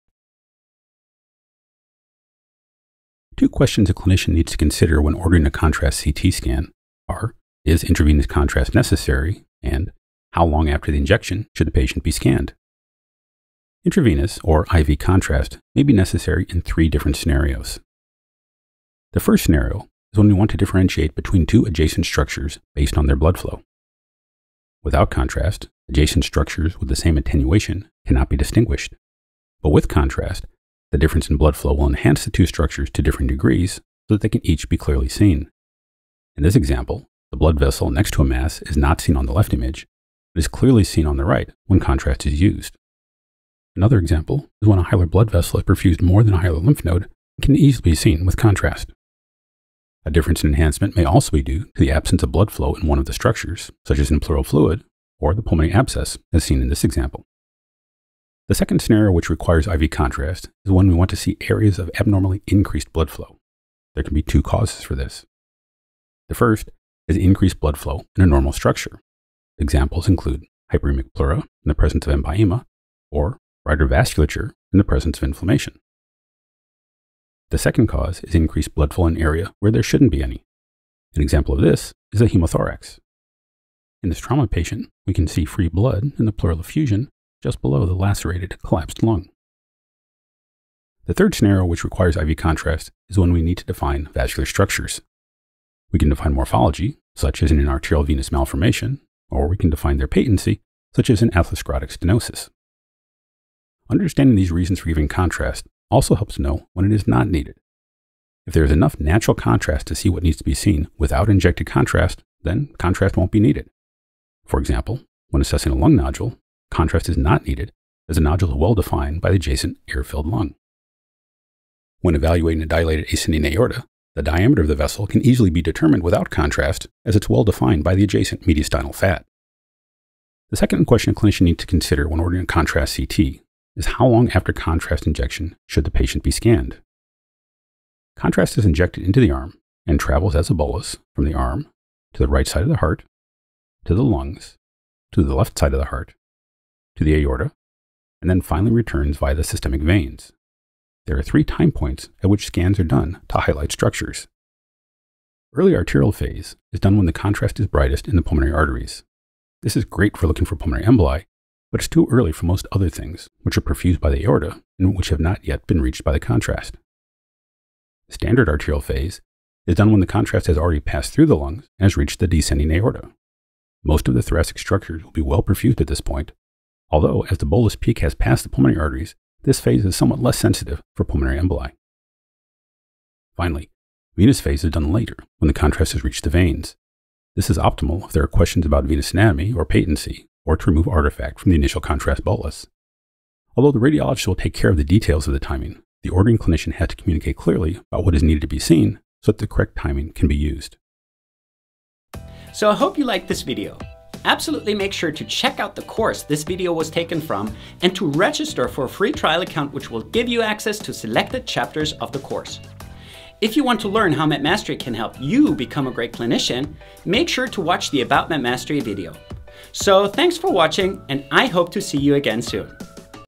When to use intravenous contrast for chest CT
There are two questions a clinician needs to consider when ordering a contrast CT scan: is intravenous contrast necessary? How long after the injection should the patient be scanned?

In this video, taken from our Chest CT Essentials course, we answer these questions and review when and how to use intravenous contrast for a chest CT.

Master this step-by-step process for diagnosing the most common cardiothoracic diseases using chest CT. Start the course now!.